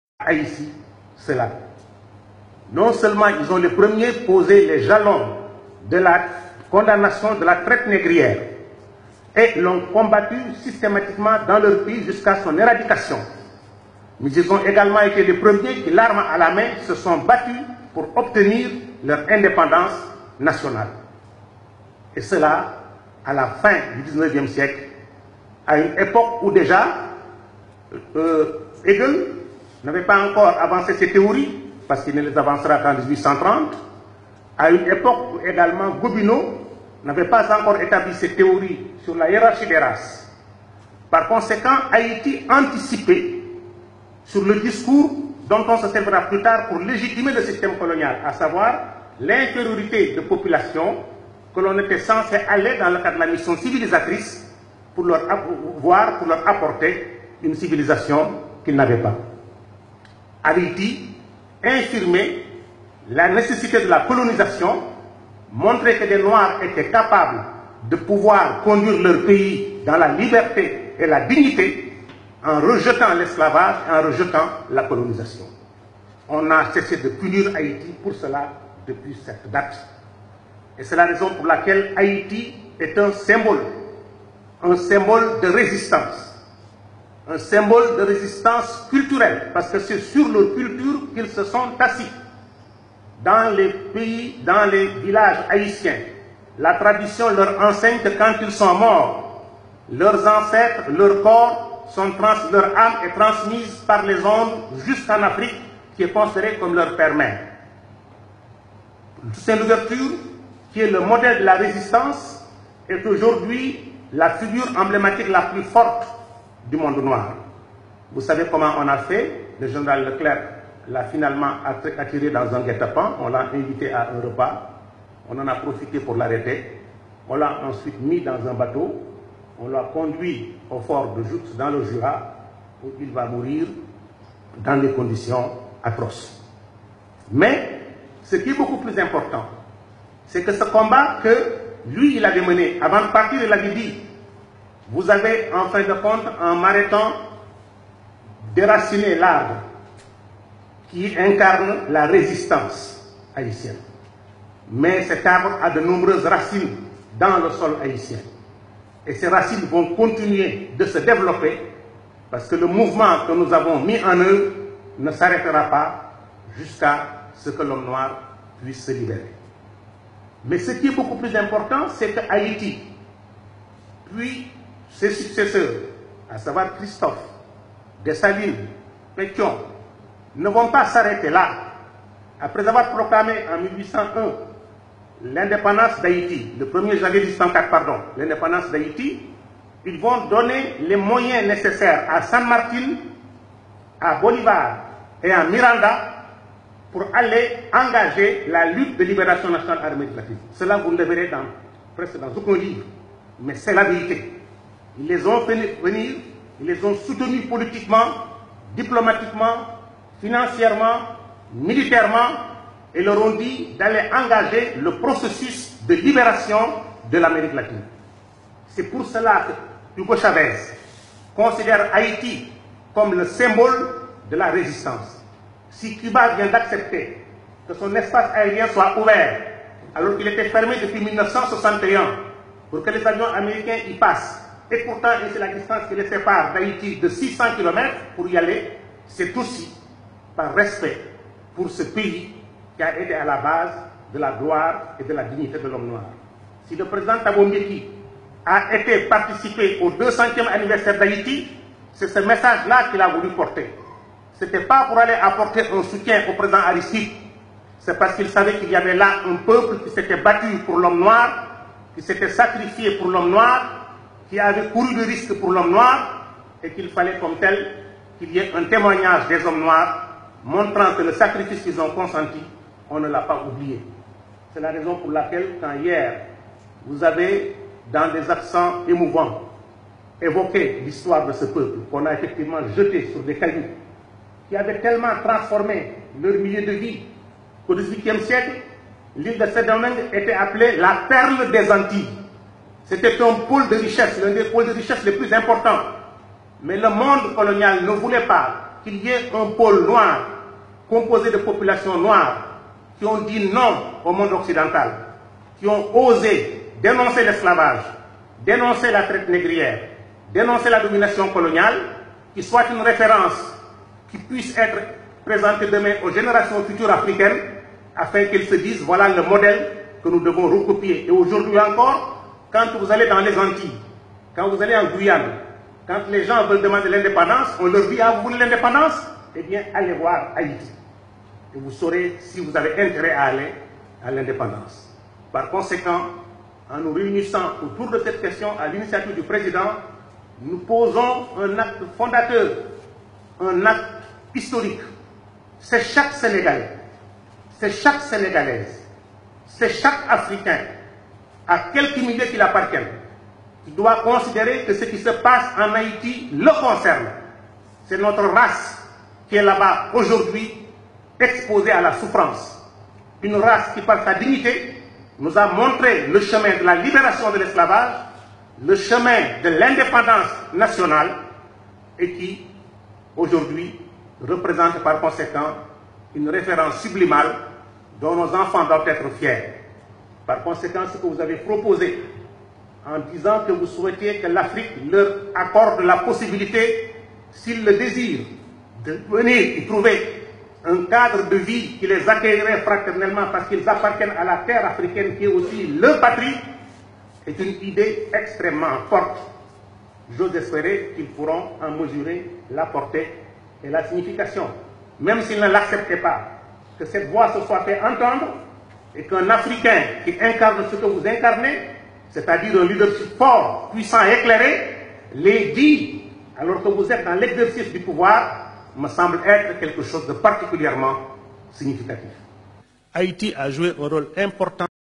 Un Sénégalais 🇸🇳 parle de l'histoire d'Haïti 🇭🇹
L'orateur s'appelle Ibader Thiam.